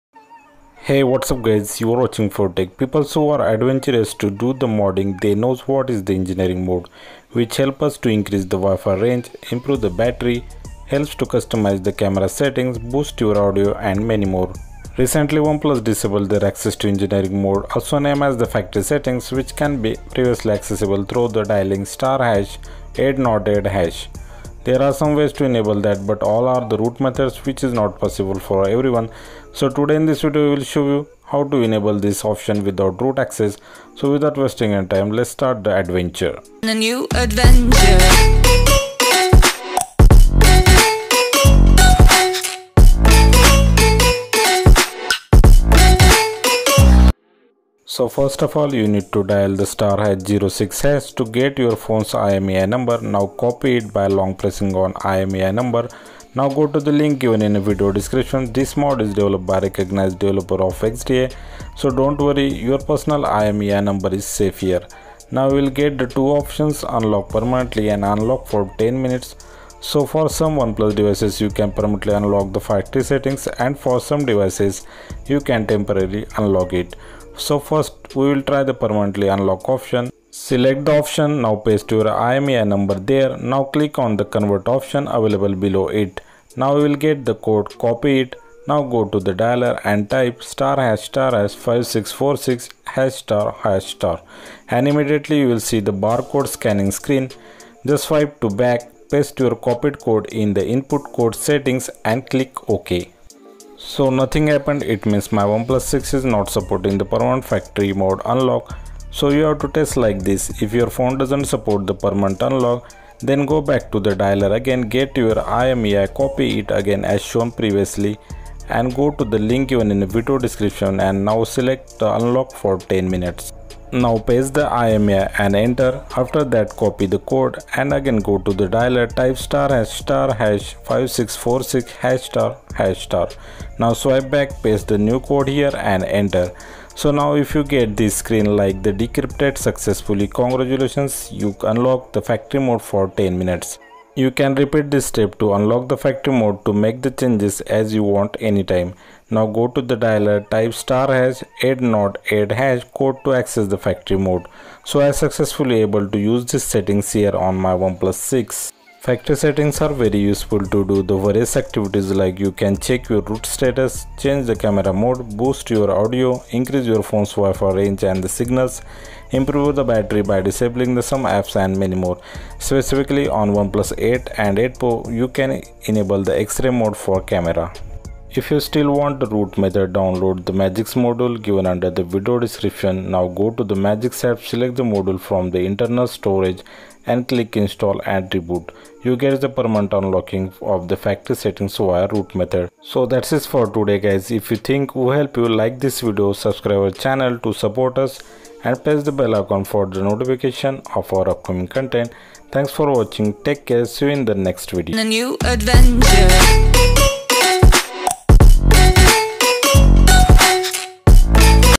Enable Engineering Mode Android/Factory Mode For Any OnePlus Phone Without Root l Latest 2021
So, OnePlus disabled access to engineering mode for all OnePlus devices recently. So you'll not be able to access it now via dialing *#808# in dialer. There are root methods but now developers found no root method so follow video till the end.

Note: People using OnePlus 8 and 8 pro can enable x-ray mode using this method just enable this mod and under camera settings of factory mode click fourth back camera preview
and take photo as photochrome filter means now you can take x-ray type pics.

Dialer codes:
1) access imei codes:

 acess factory mode/engineering mode:Nord 2
OP Feedback / Bug Report = *#800#
Engineering mode = *#899#

3) access bar code mode to enter copied code to unlock factory mode: